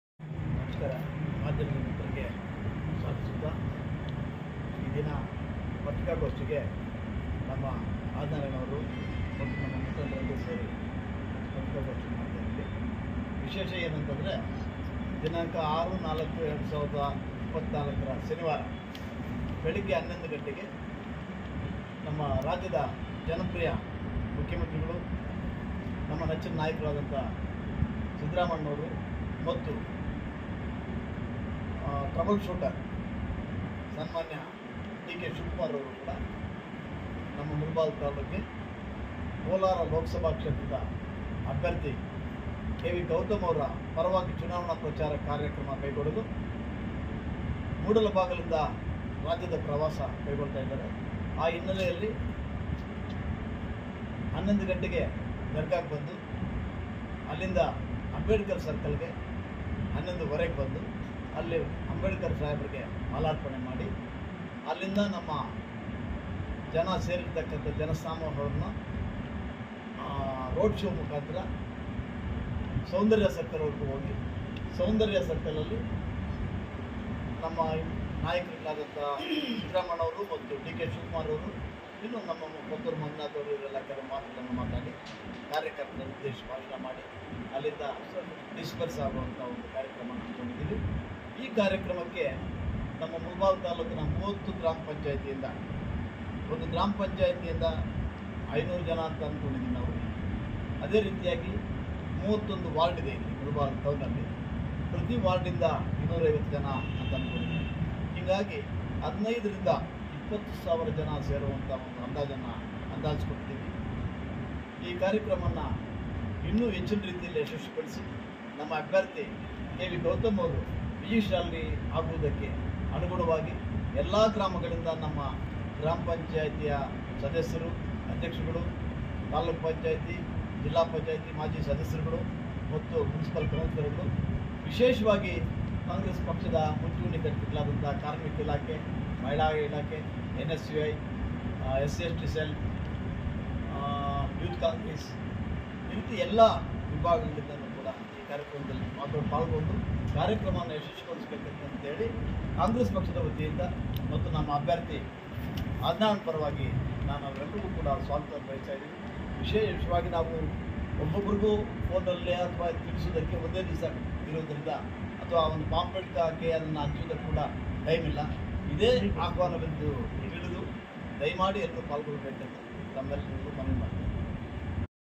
ಮುಳಬಾಗಿಲಿನಲ್ಲಿ ಸಿಎಂ ಸಿದ್ದು ಡಿಕೆಶಿ ರವರಿಂದ ರೋಡ್ ಶೋ ಬ್ಲಾಕ್ ಅಧ್ಯಕ್ಷ ಎಂಸಿ ನೀಲಕಂಠೇಗೌಡ ಸರ್ವರಿಗೂ ಸುಸ್ವಾಗತ
ಲೋಕಸಭೆ ಕಾಂಗ್ರೆಸ್ ಅಭ್ಯರ್ಥಿ ಕೆವಿ ಗೌತಮ್ ಪರ  ಪ್ರಚಾರಕ್ಕೆ ಸಿಎಂ ಸಿದ್ದರಾಮಯ್ಯ ಮತ್ತು ಉಪ ಮುಖ್ಯಮಂತ್ರಿ ಡಿಕೆ ಶಿವಕುಮಾರ್ ರವರು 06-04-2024 ರಂದು ರೋಡ್ ಶೋಗೆ ಆಗಮಿಸಲಿದ್ದು ರೋಡ್ ಶೋ  ಹೆಚ್ಚಿನ ಪ್ರಮಾಣದಲ್ಲಿ ಸಮಸ್ತ ನಾಗರೀಕರು ಆಗಮಸಲು ಮನವಿ ಮಾಡಿದ ಬ್ಲಾಕ್ ಕಾಂಗ್ರೆಸ್ ಅಧ್ಯಕ್ಷ ಎಂಸಿ. ನೀಲಕಂಠೇಗೌಡ